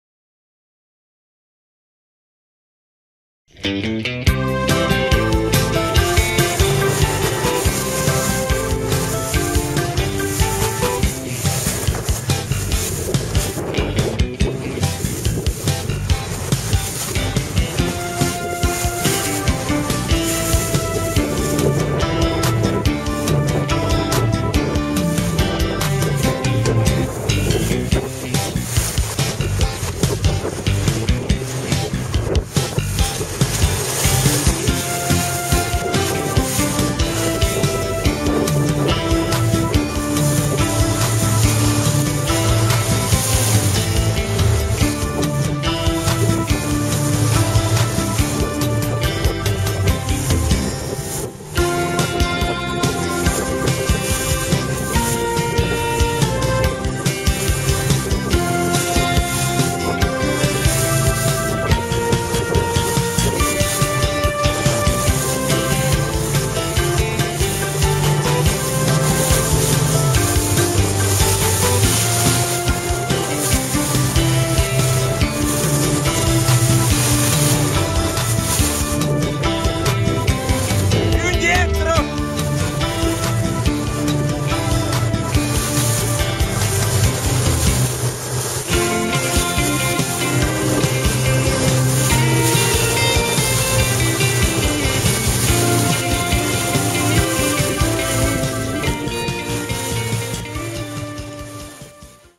Ever seen a Star so Fast? Diego Negri & Enrico Voltolini in Loano
Italian Star Class sailors of Fiamme Gialle (World Ranking #2), Diego Negri and Enrico Voltolini, training in Loano with 40 KTS.